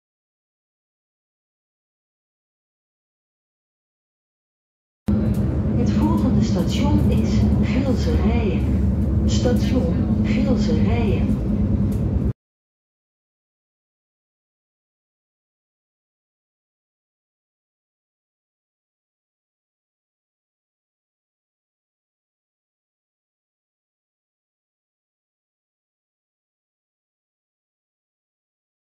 NS station announcement (in train); Gilze-Rijen station (20240123)
NS Afroep in trein voor aankomst station Gilze-Rijen.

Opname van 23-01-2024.